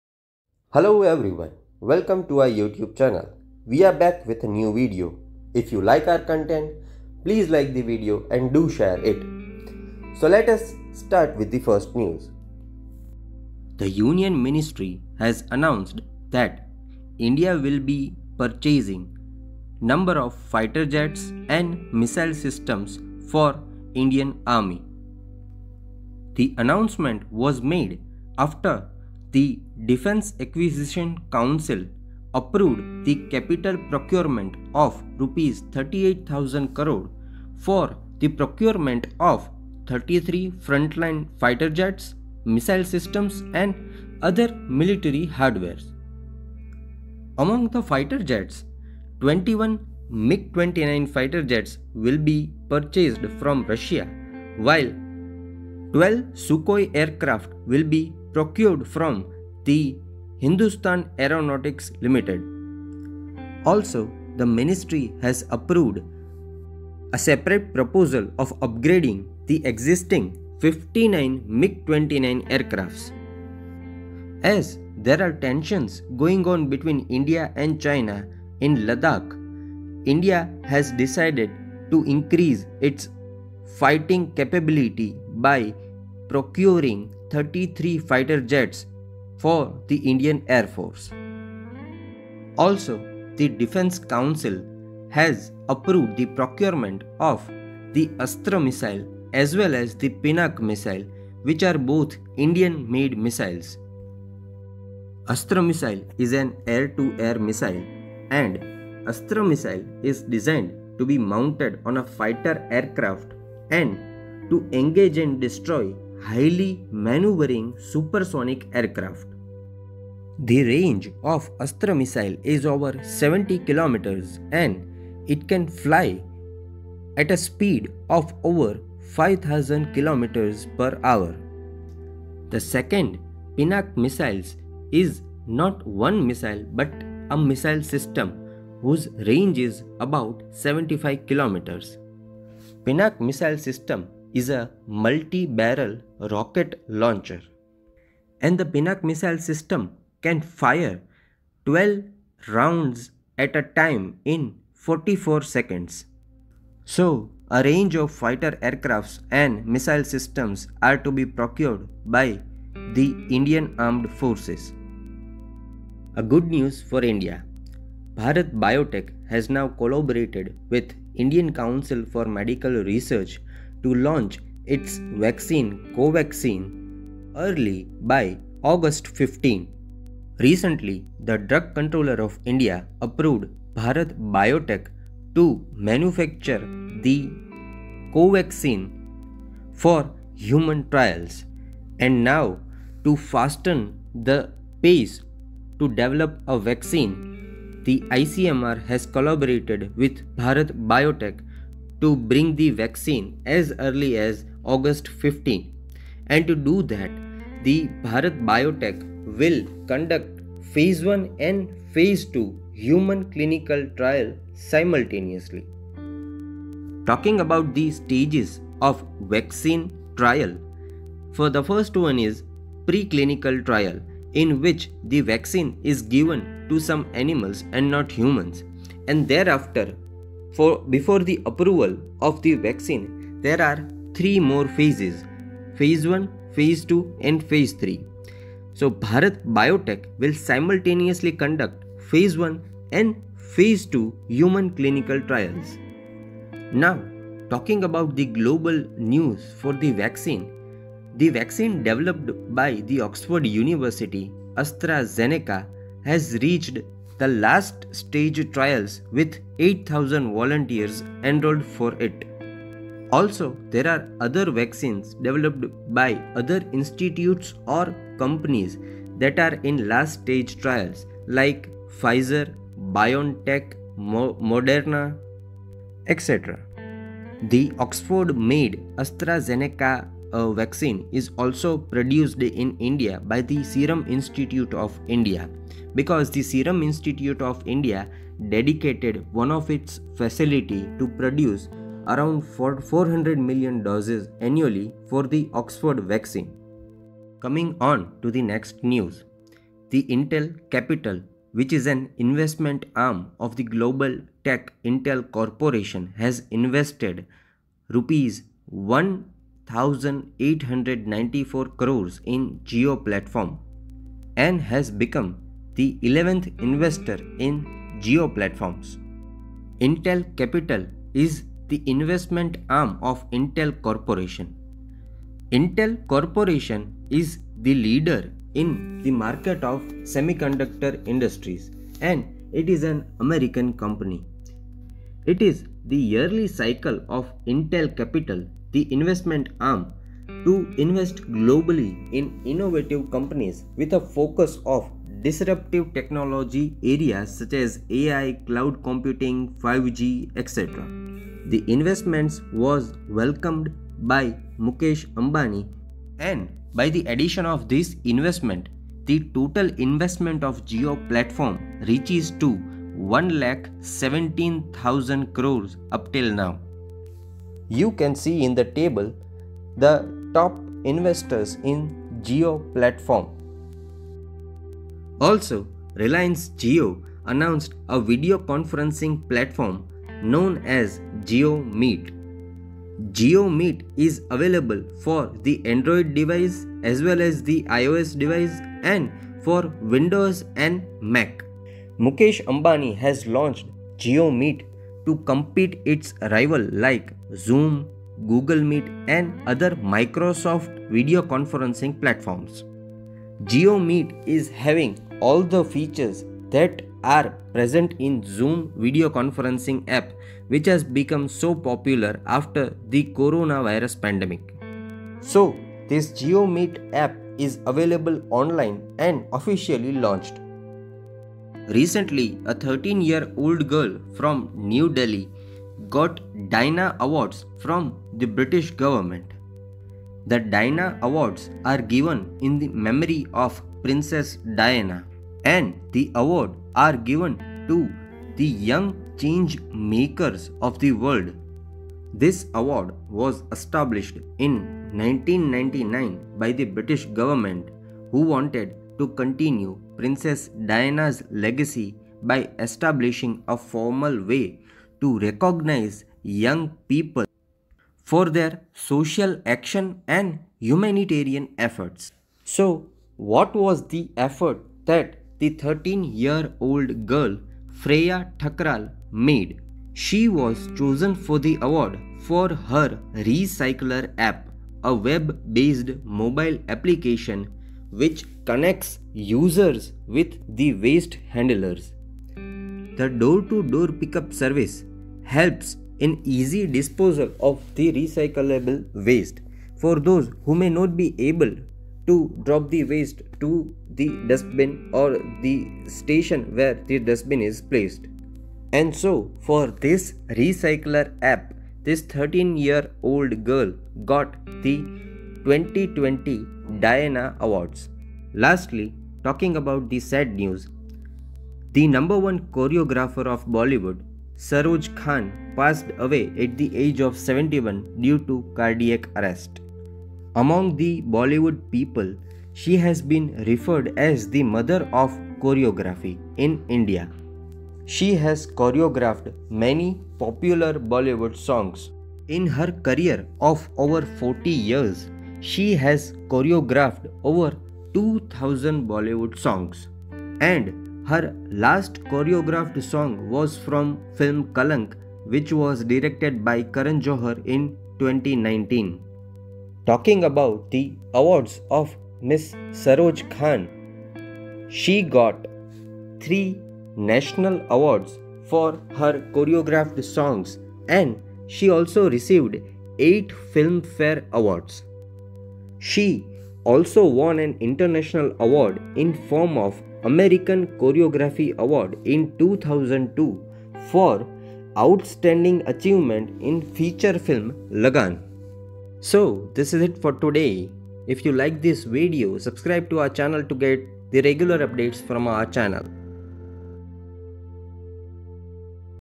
Fighter Jets | Missiles | Jio Meet | Covaxin | Intel Capital | Saroj Khan | The Diana Awards
Content :

1. India to acquire 33 fighter jets and missiles : 00:14
2. Covaxin to be ready by August 15 : 02:46
3. Intel Capital becomes 11th investor in Jio : 04:57
4. Delhi girl bags Diana Award: 07:01
5. Saroj Khan passes away : 08:29